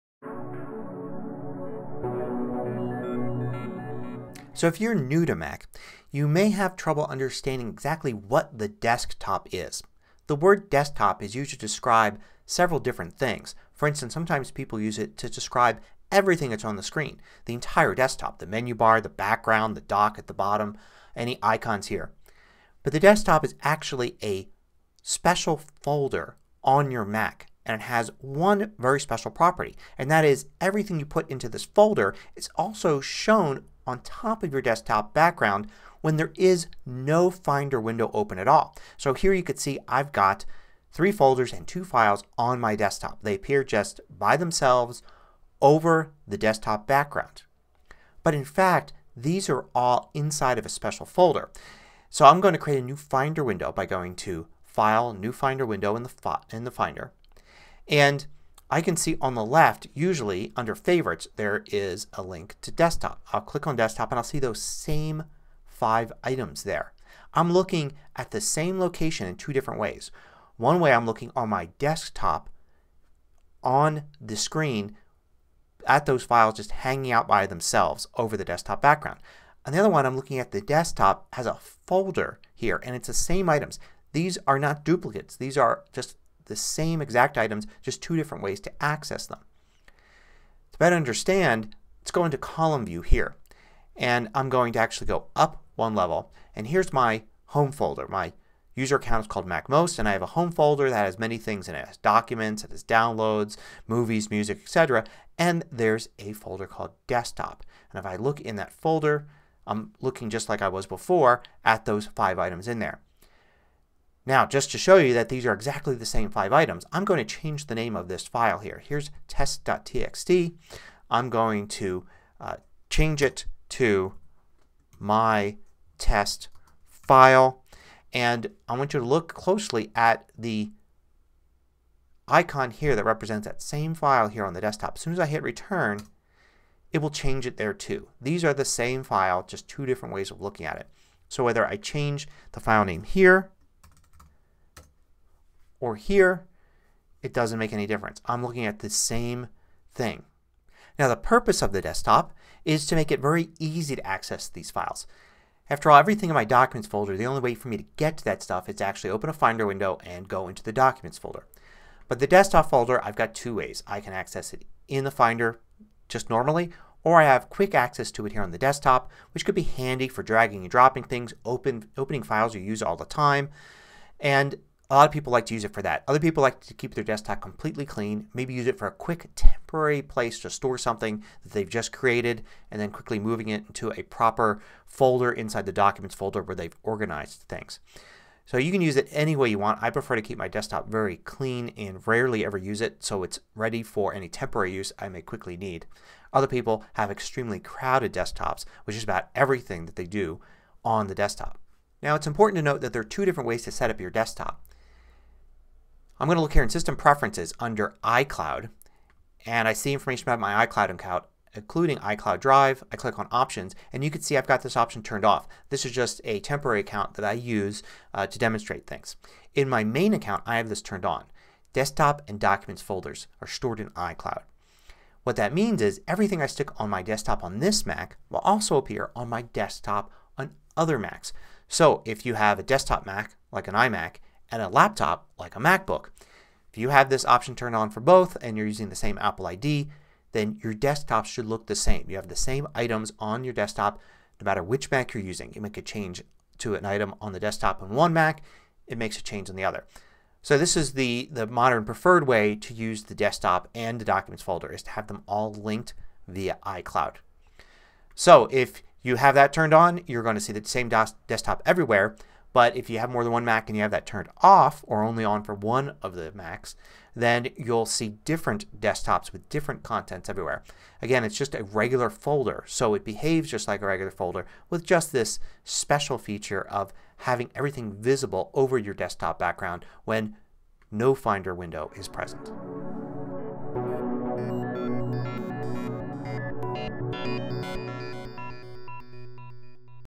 A Beginner's Guide To the Desktop Folder (#1581)
The Desktop folder is just a regular folder with the special property of existing over your desktop background as well as in Finder windows. Items on your Desktop are the same items as the ones you see in the Desktop folder in your Home user folder. The Desktop can be useful for quick or frequent access to files and folders, although many prefer to keep the Desktop clean and empty or use it for temporary storage of new files only.